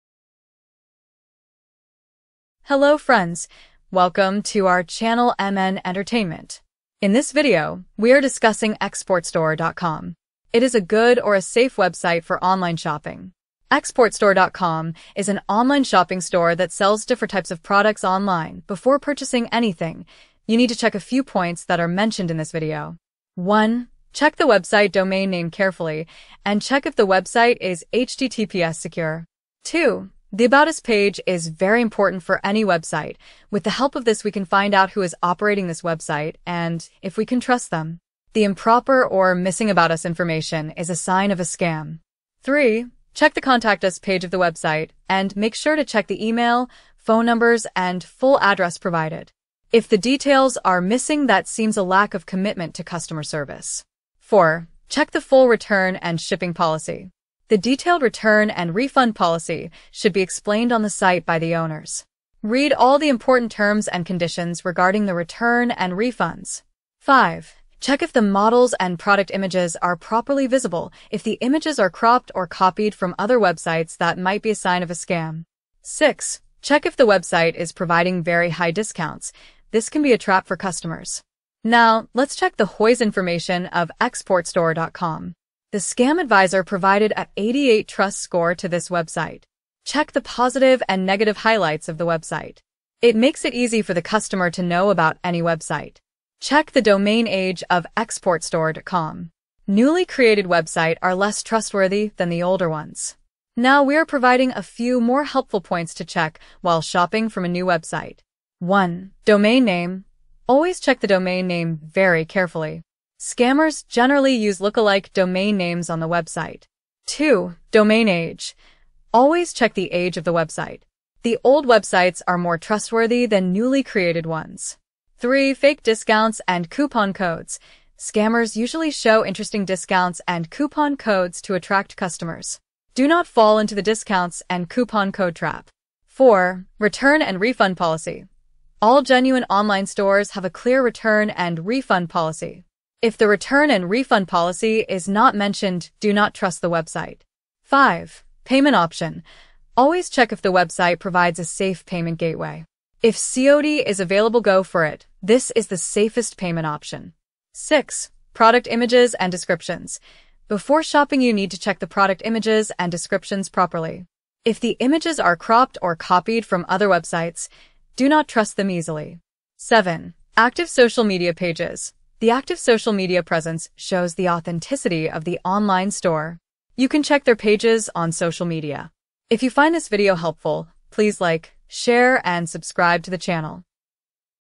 Xportstore Review | Is Xportstore.com a Scam or Legit Website?
If you have any questions, email or write them down in the comment section.